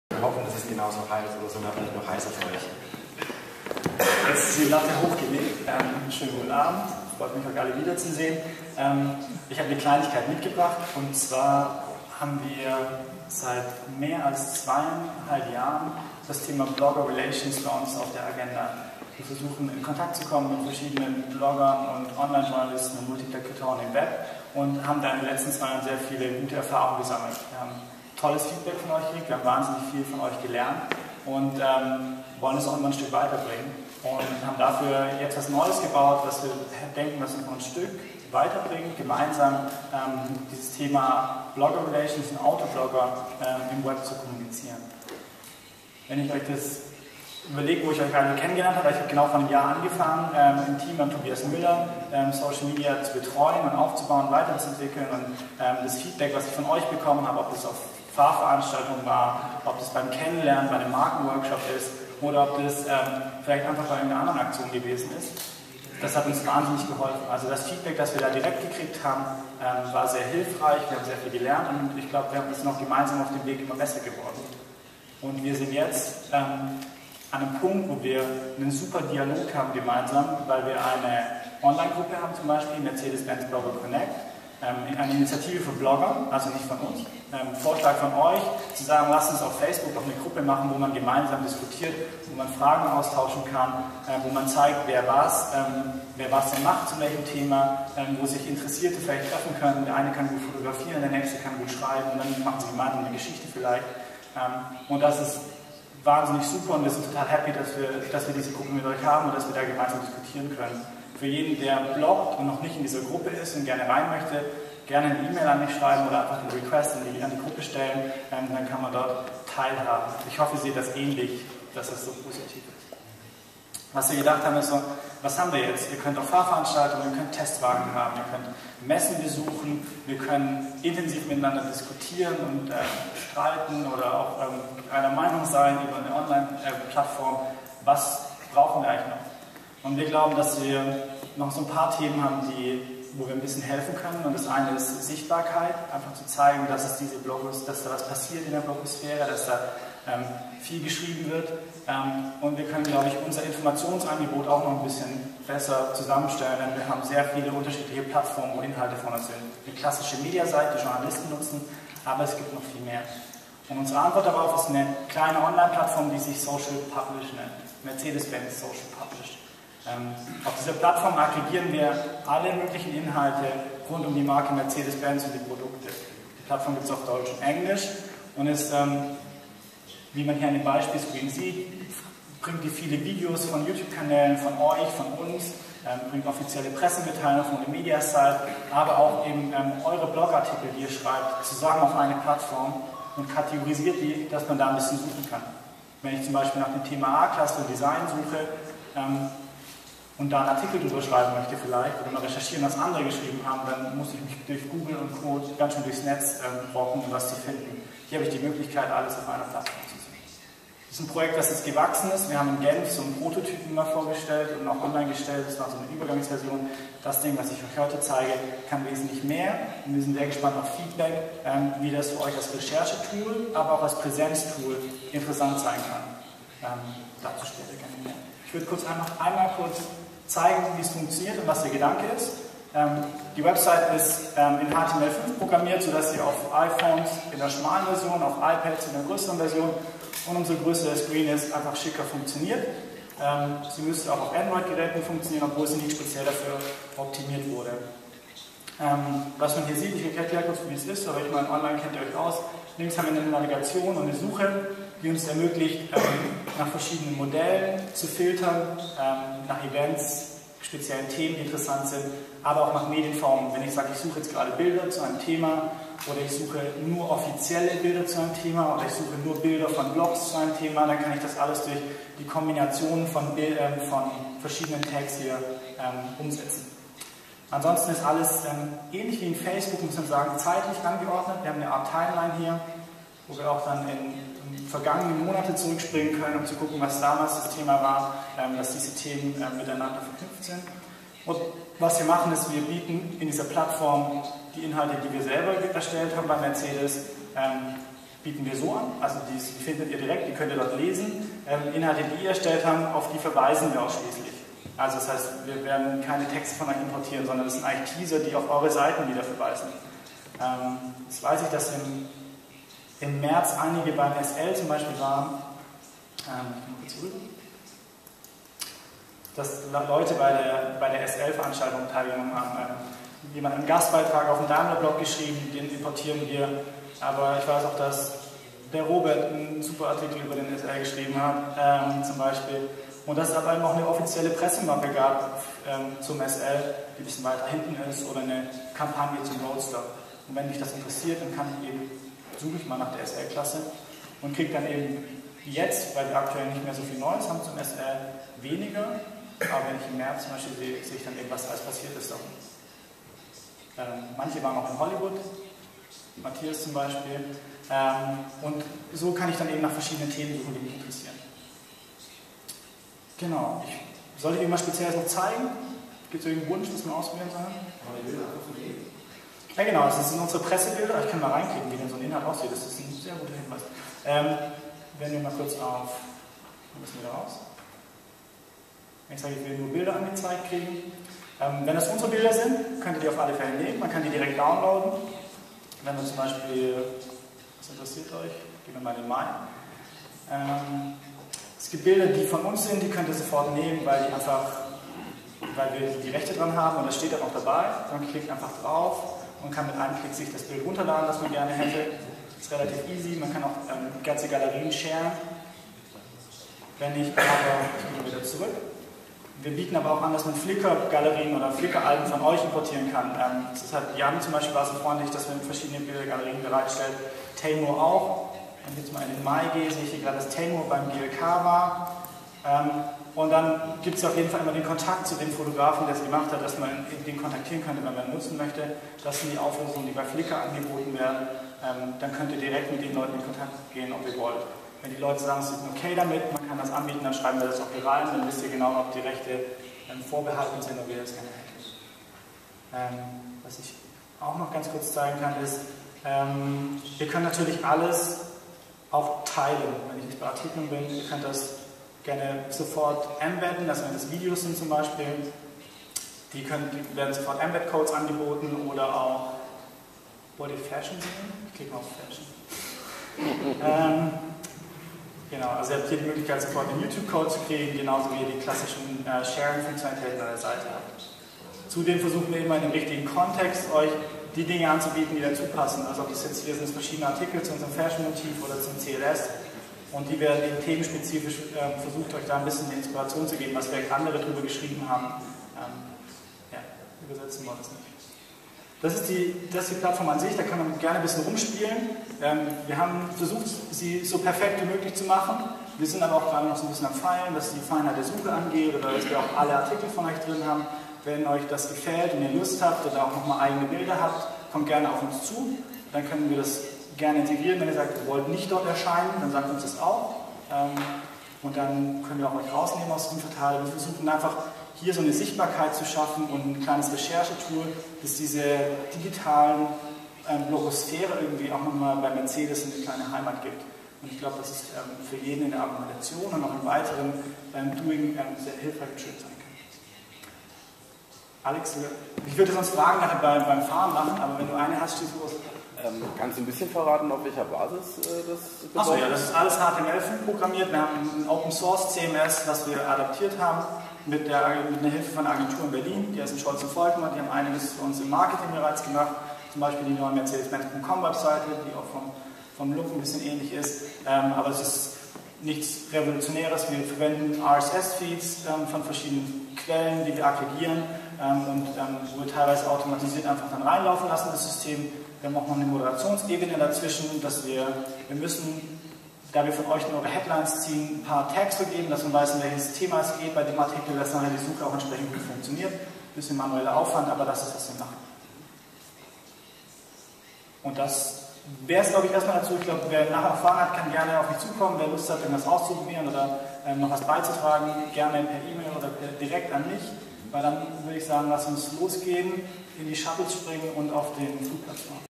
Mercedes-Benz Social Publish
Wolfgang Würth (von MB) erklärt die neue Plattform Social Publish.